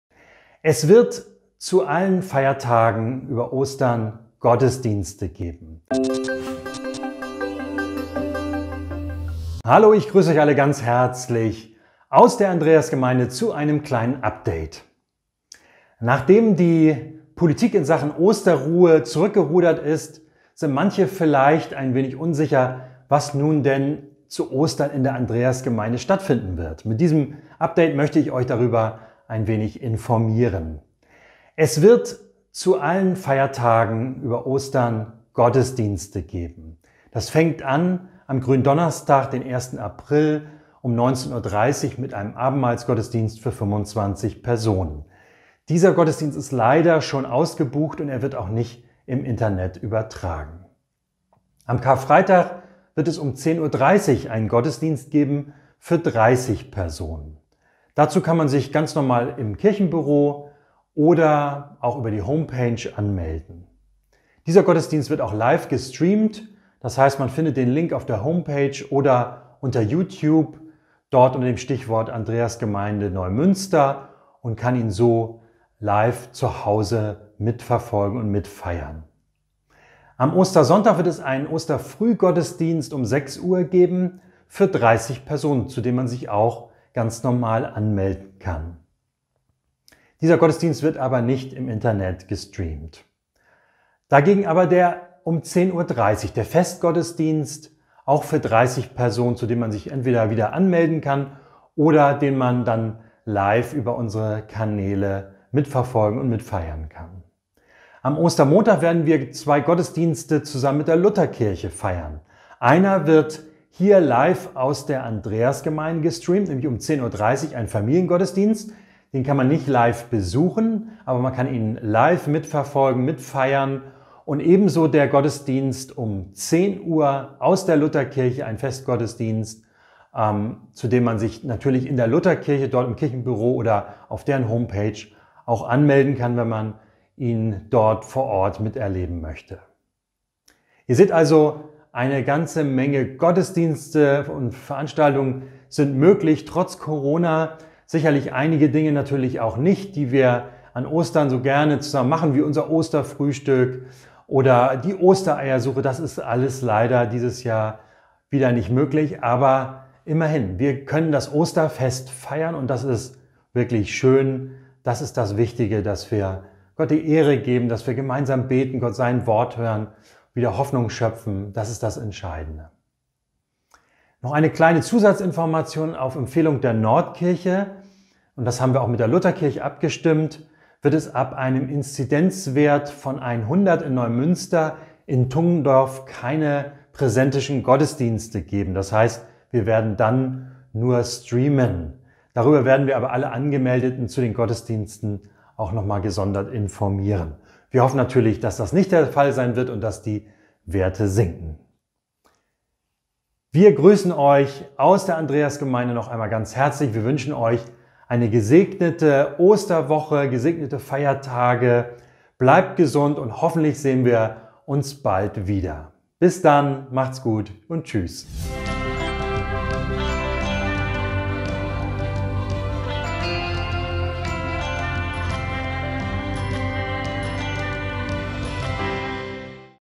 Gottesdienste an Ostern | Update vom 27. März 2021
Alle Gottesdienste an Ostern:

GRÜNDONNERSTAG
✝️ Abendmahlsgottesdienst
➡️ 19:30 Uhr
➡️ vor Ort mit Anmeldung

KARFREITAG
✝️ Gottesdienst
➡️ 10:30 Uhr
➡️ Online und vor Ort mit Anmeldung

OSTERSONNTAG
✝️ Frühgottesdienst
➡️ 6:00 Uhr
✝️ Festgottesdienst
➡️ 10:30 Uhr

OSTERMONTAG
✝️ Familiengottesdienst
➡️ 10:30 Uhr
➡️ Online
✝️ Festgottesdienst
➡️ 10:00 Uhr
➡️ in der Lutherkirche